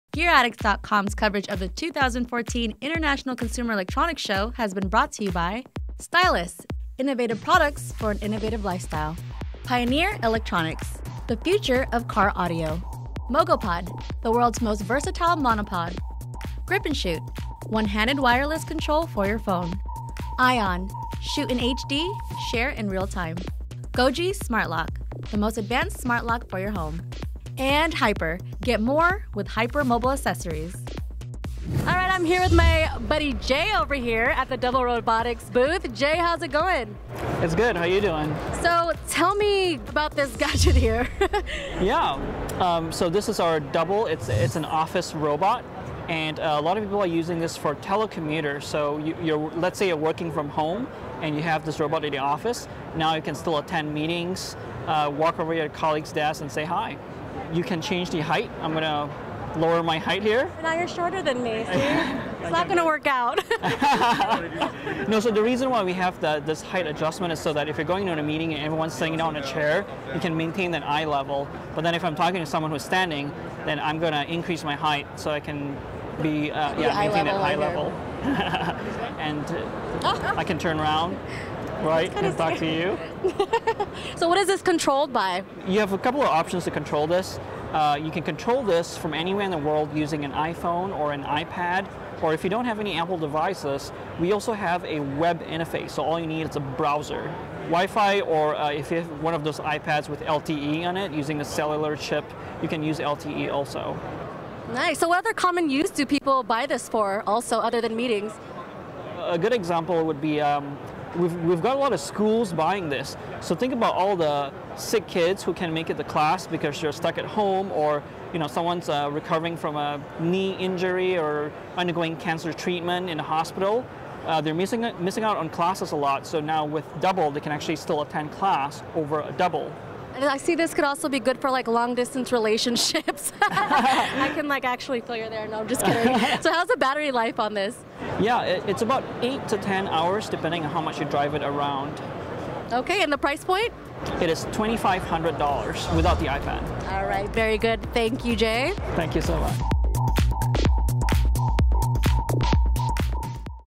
Double Robotics "Double" Telepresence Robot (CES 2014)
Keiko Alingas stops by the Double Robotics booth to check out their "Double" Motion Controlled Telepresence Robot.